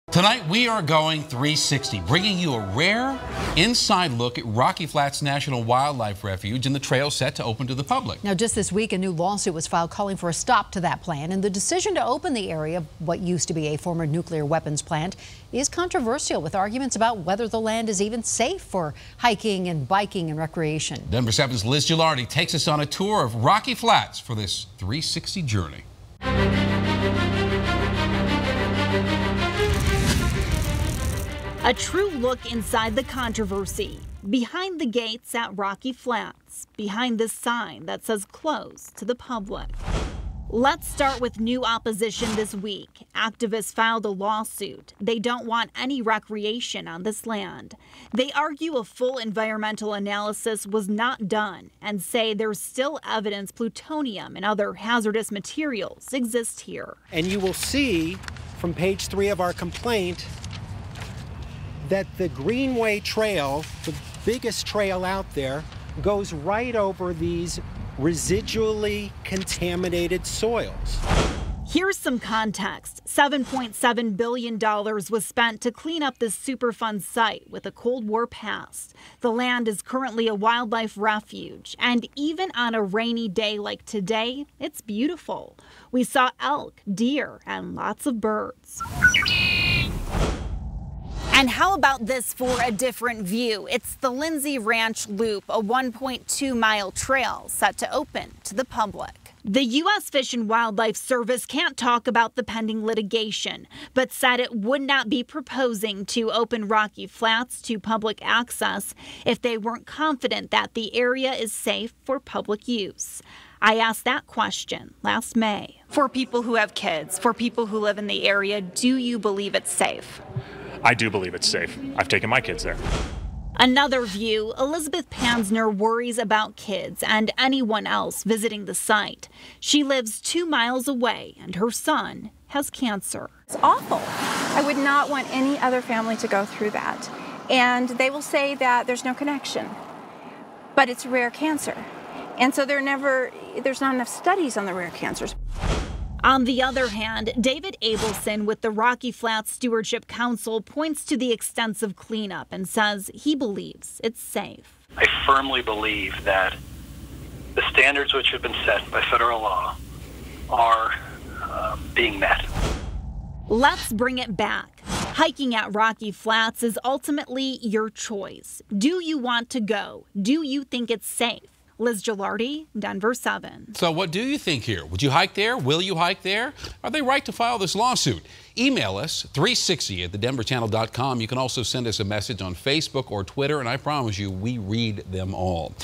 A rare look inside Rocky Flats as new lawsuit challenges opening of public trails
As a new lawsuit is pending Denver7 is take a rare look inside Rocky Flats National Wildlife Refuge and the trails set to open to the public.